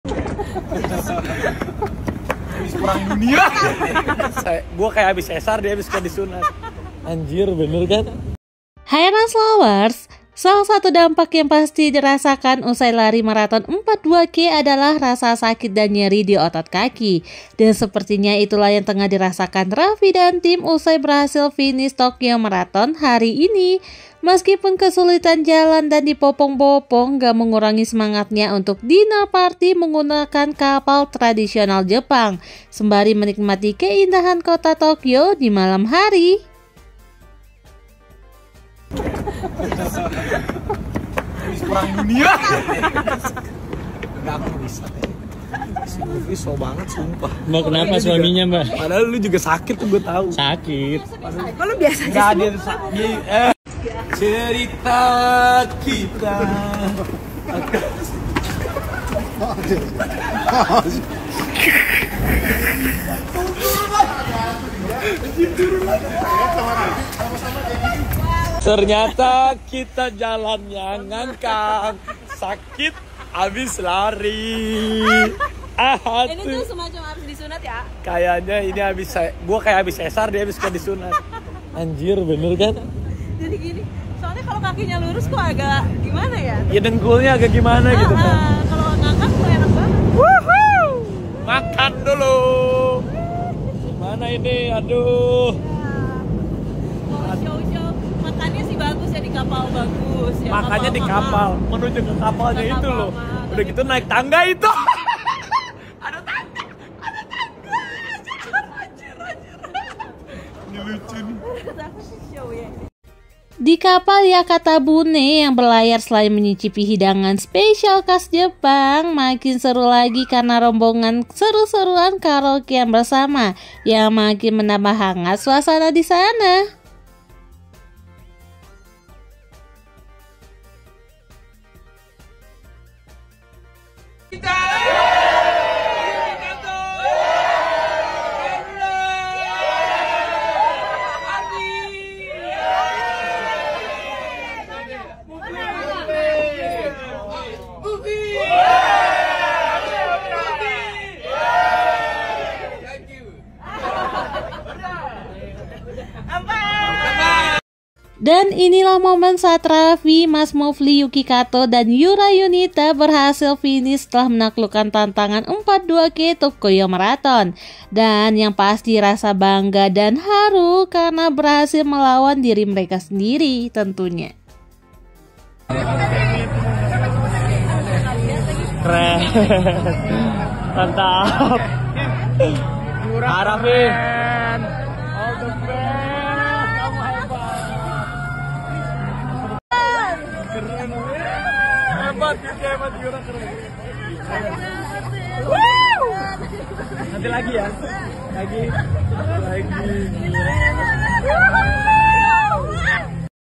TAHAN SAKIT SAMPAI JALAN DI BOPONG2, RAFFI NAGITA & TIM GELAR DINNER PARTY DI KAPAL MEWAH RATUSAN JT
video terbaru Rans edisi Raffi Nagita Rayakan kemenangan bersama Yura Yunita Yuki Kato dan Rombongan usang berhasil Finish Strong di Tokyo Marathon, Dinner mewah di Kapal Tradisional Jepang